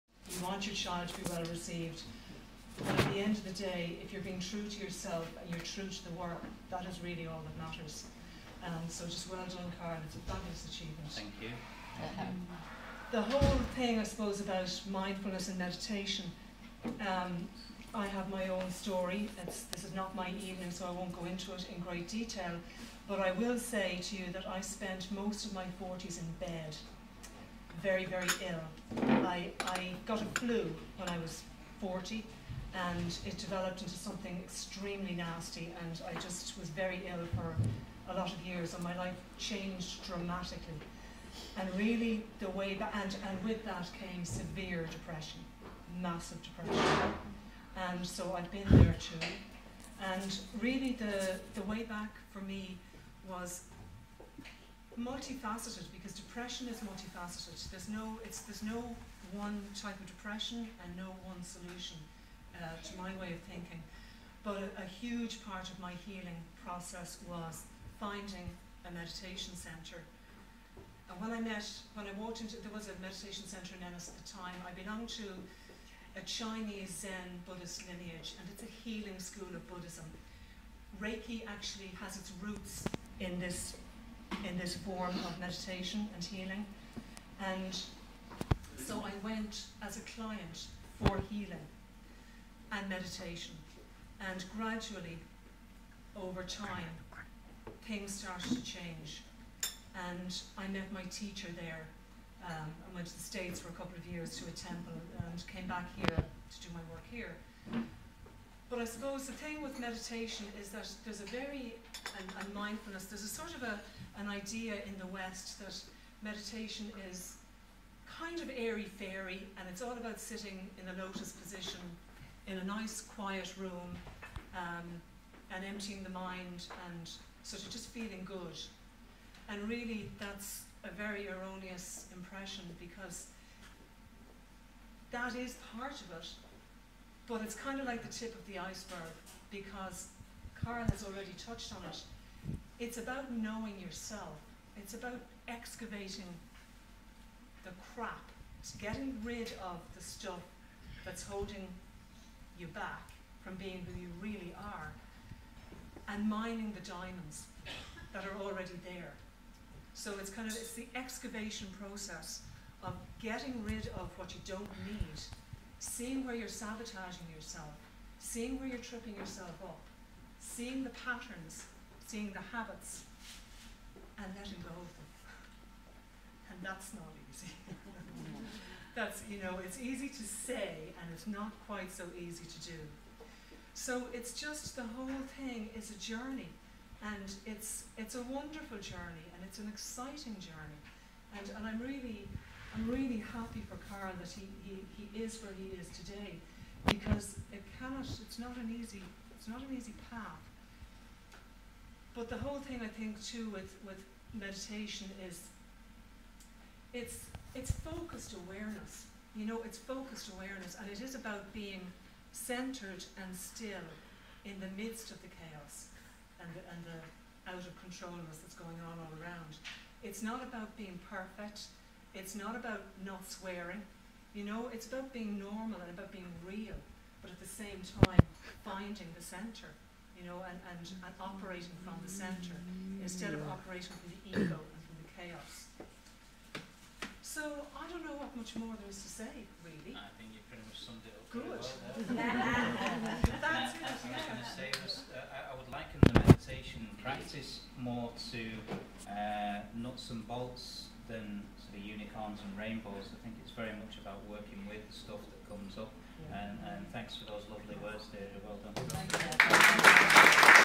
Clare Book Launch "The Fox and the Scorpion meet Cold Turkey" by Carl J Ashley
Friday 11th March 2016 saw the inaugural book launch of debut author Carl J Ashley at Suas Coffee House in O'Connell St, Ennis, Co.Clare. Guest speaker from Clare Meditation Deirdre Devally kindly said a few words about one of the themes in the book - using a mindfulness and meditation practice to help deal with depression. for more information. Thankfully, the informal, evening atmosphere was enjoyed by a collection of esteemed friends and family, and it was a lovely occasion. Thanks to everyone who attended!